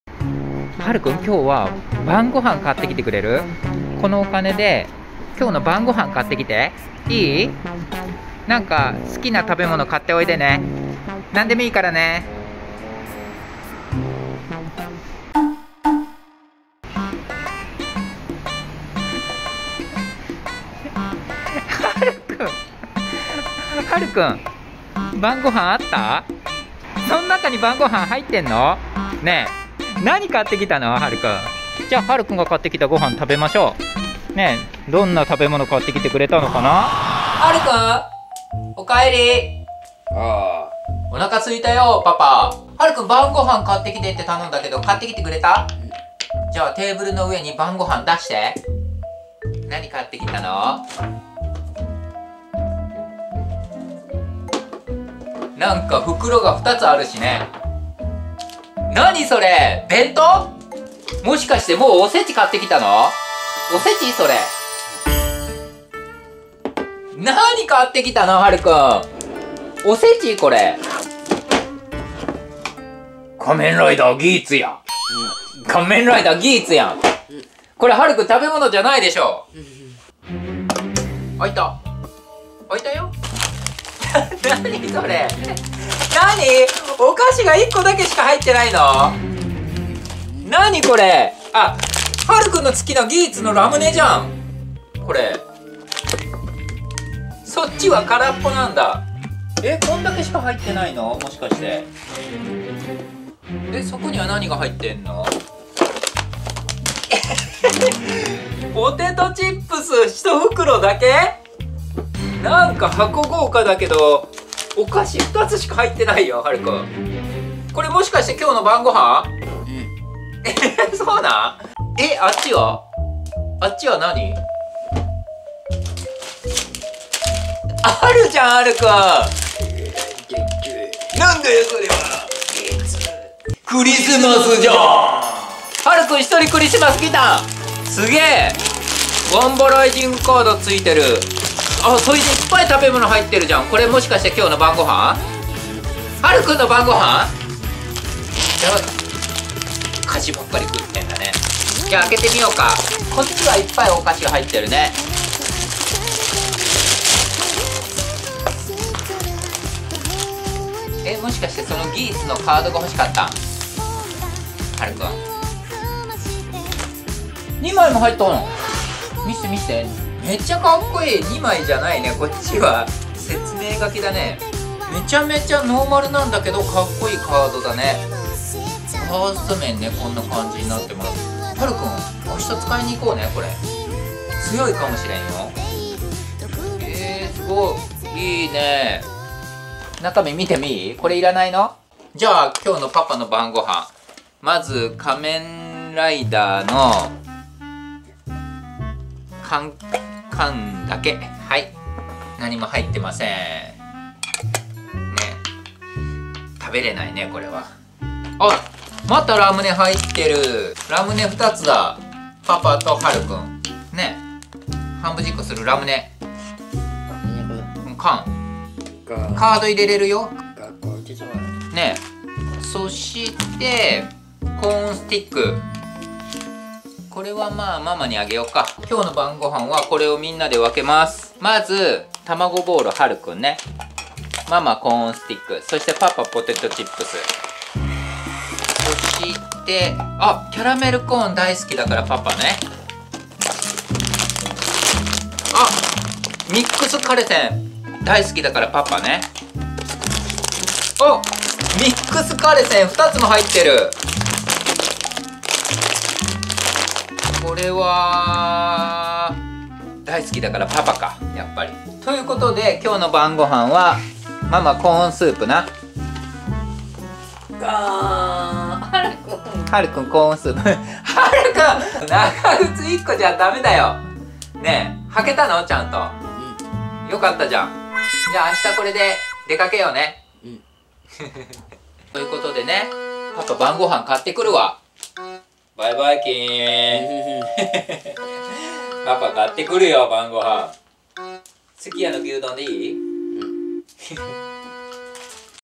はる君にお使いを頼んだら？
今回は晩御飯を買ってきてとはるくんに頼みました、果たして何を買ってくるかな？